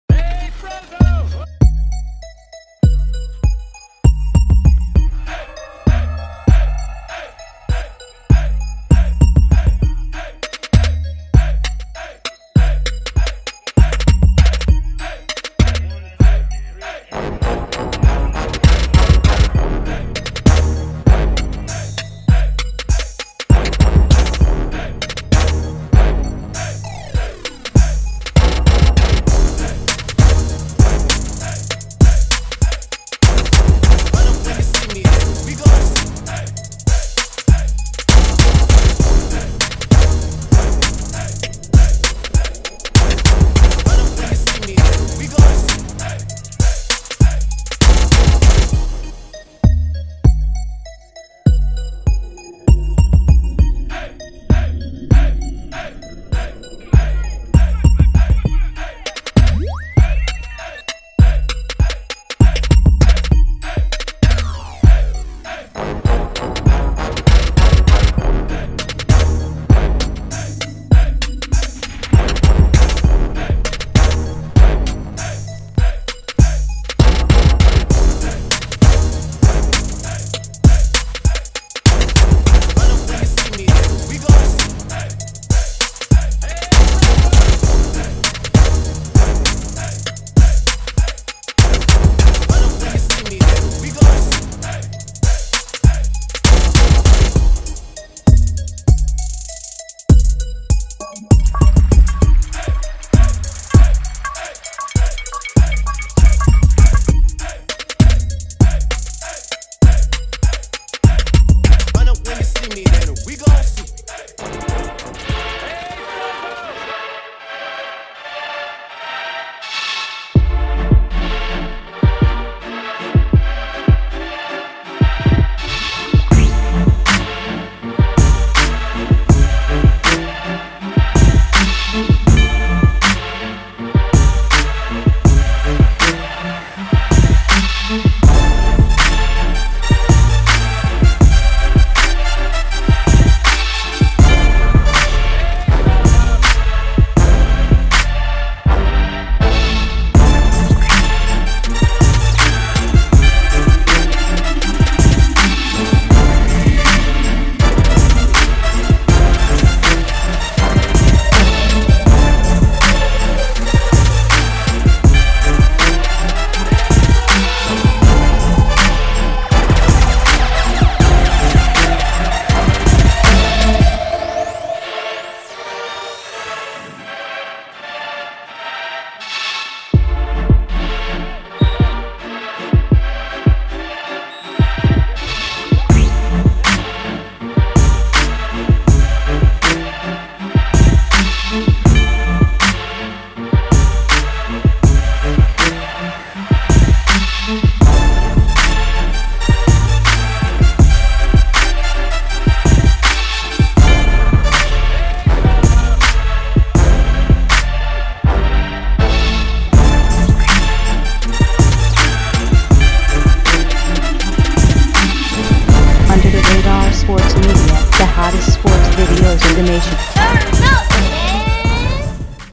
Beau Green (Turlock, CA) 7th Grade Quarterback : Kid is a Straight Baller - Spotlight 2017
🔥🔥 Beau Green - 7th Grade Quarterback : Kid is Straight Baller - Spotlight 2017